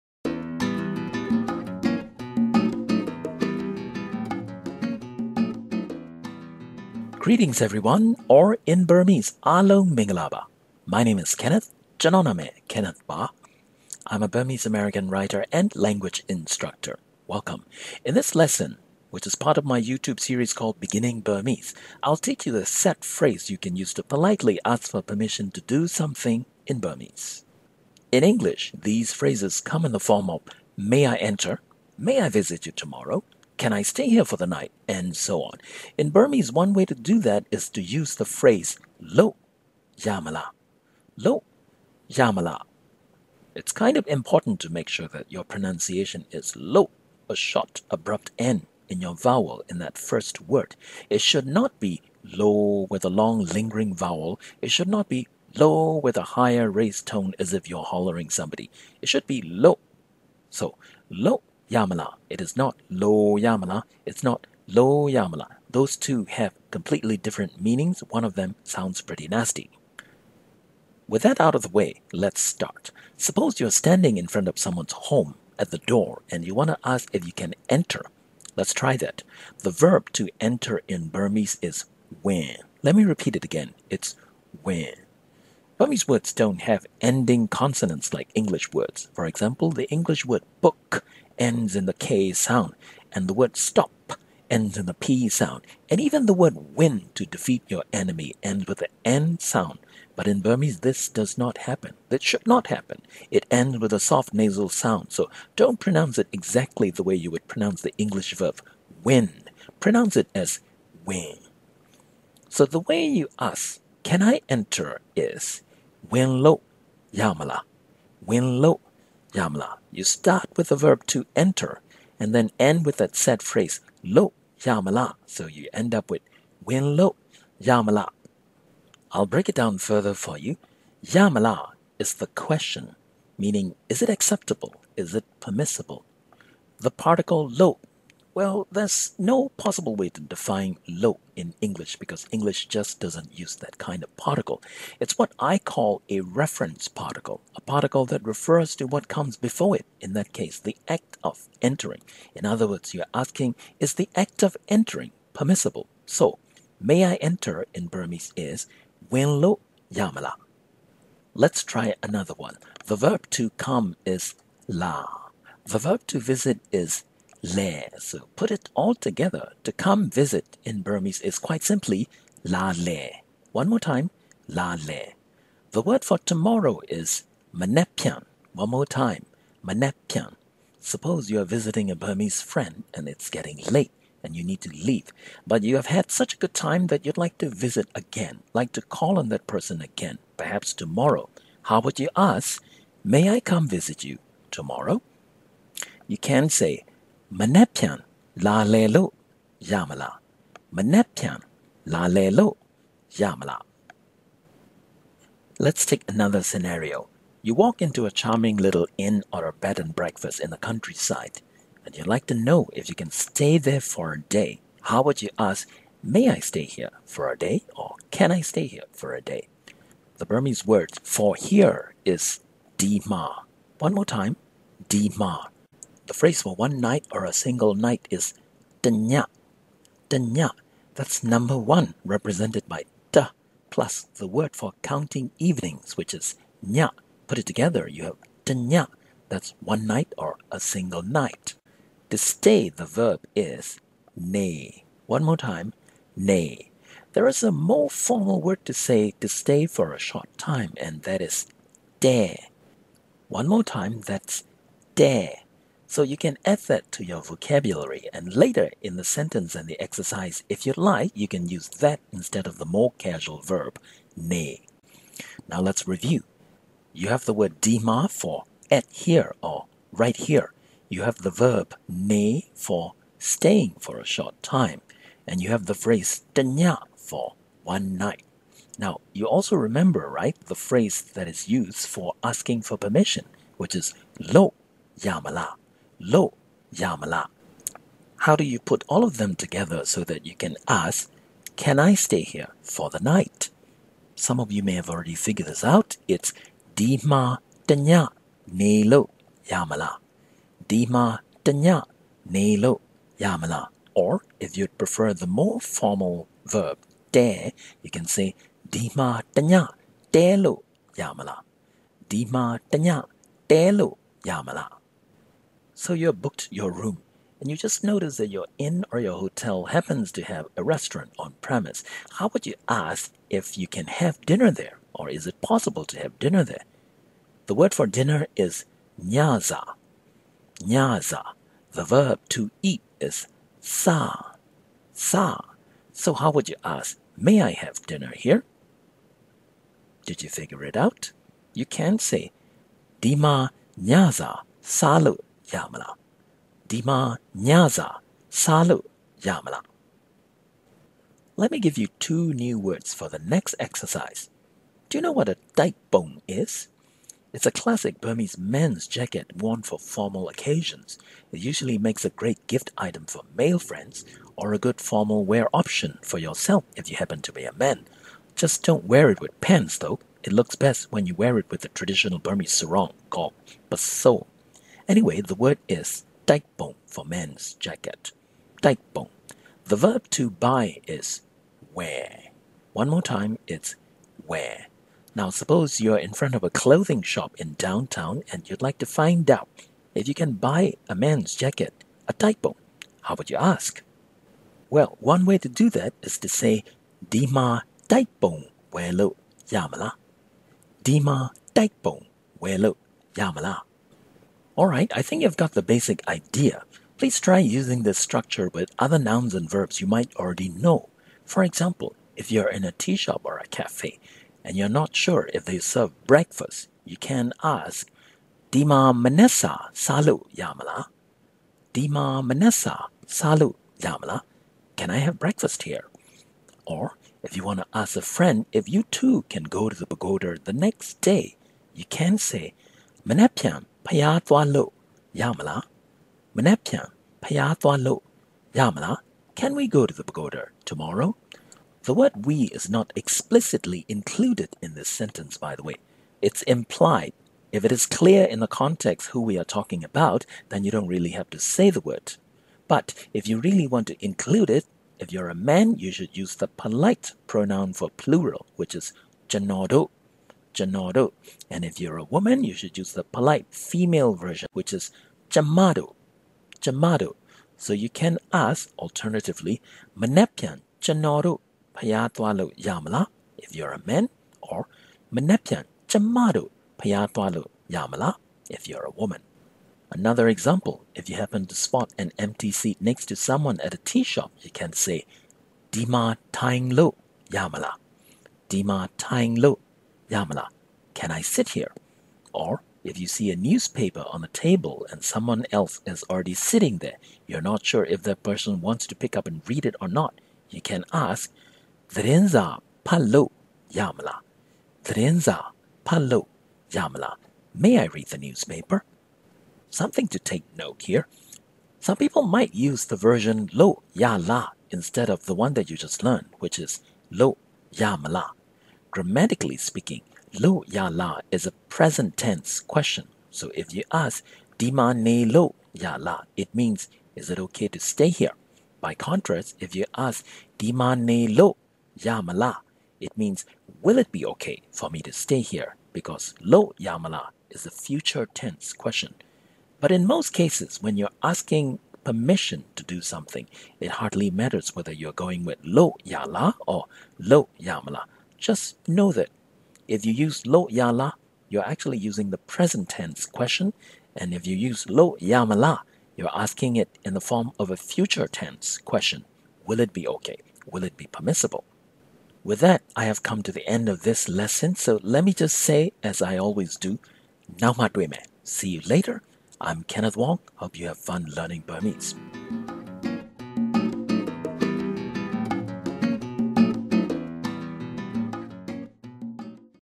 Beginning Burmese lesson- asking for permission
How to ask for permission to do something in Burmese?
For example: May I enter? May I stay here for the night? May I sit here?

Part of the Beginning Burmese lesson series by Burmese language instructor and writer Kenneth Wong

Kenneth Wong is the author of Survival Burmese Phrasebook & Dictionary (Tuttle Publishing, 2018), and A Prayer for Burma (Santa Monica Press, 2007).